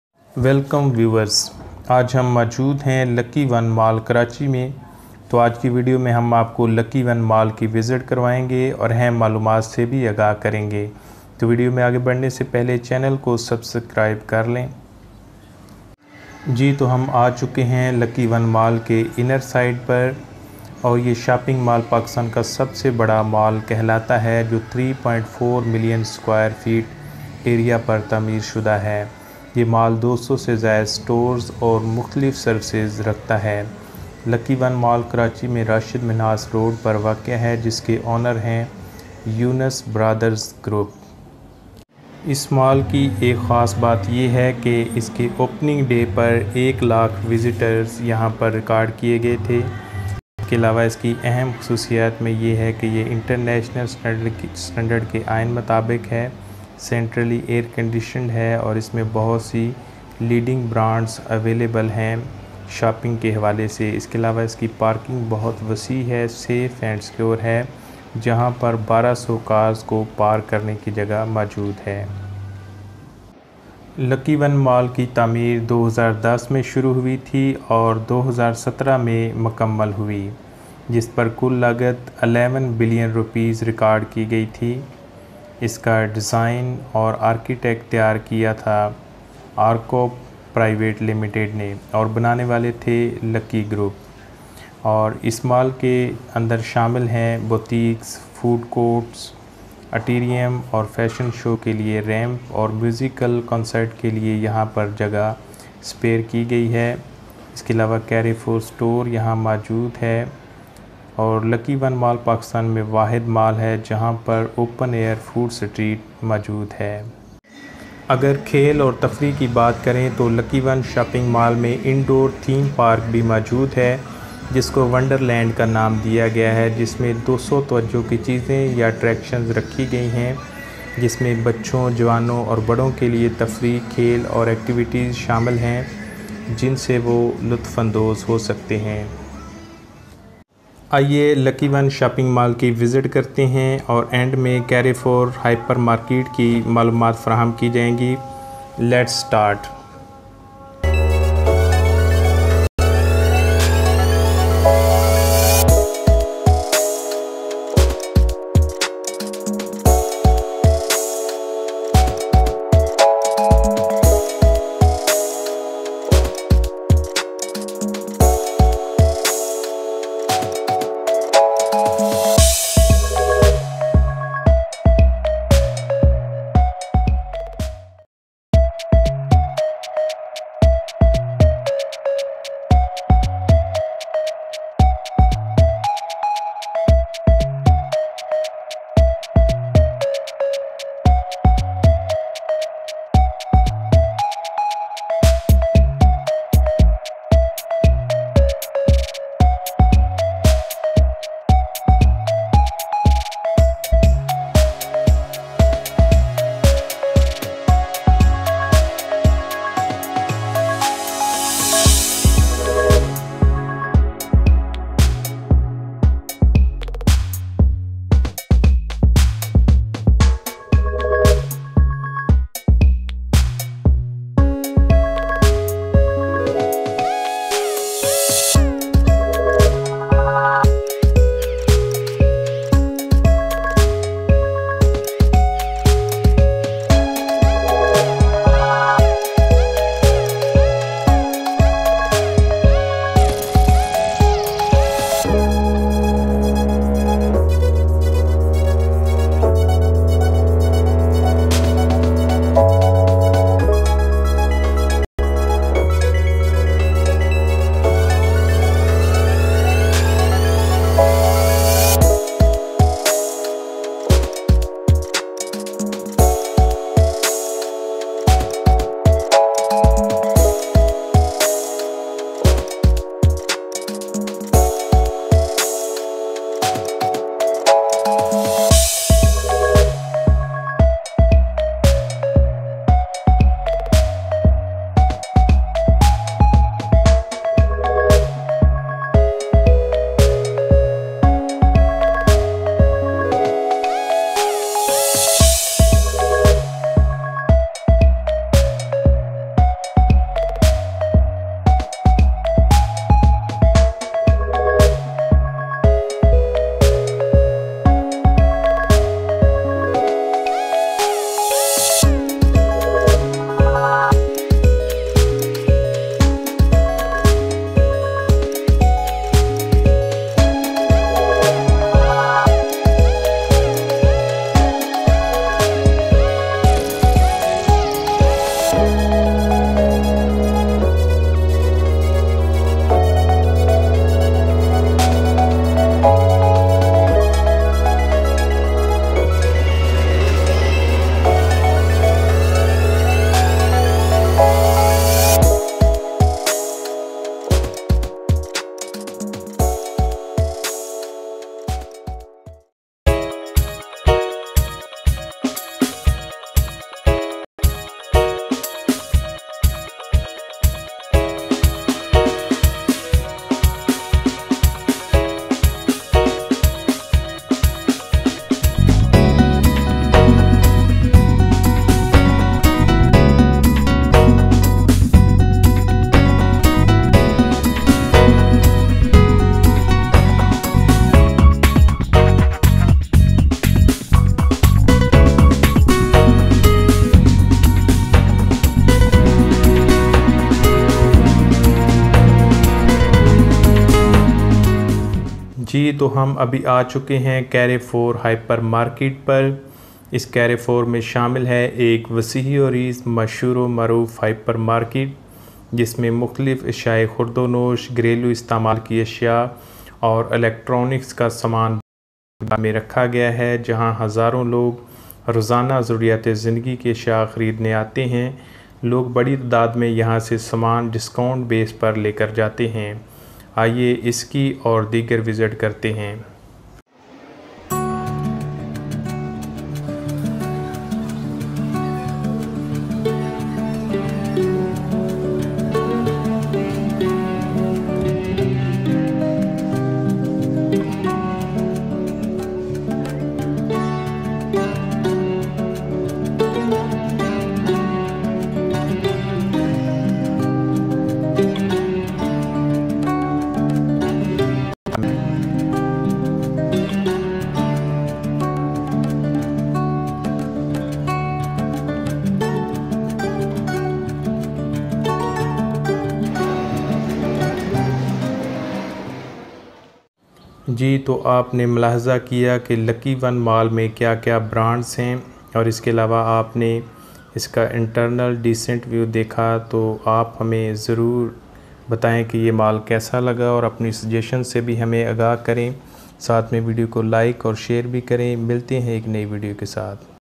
LuckyOne Mall Karachi Pakistan| Eid special on LuckyOne shopping mall
Hello friends,
In this video you can know the basic information about LuckyOne Mall Karachi,which is the largest shopping mall of Pakistan. It is a beautiful and decent environment Mall. It's Construction completed in 2017 and now open for every one. Carrefour hyper market also situated inside the Mall which has all house hold items.
Follow link
I shares videos on YouTube regarding top news and technology related.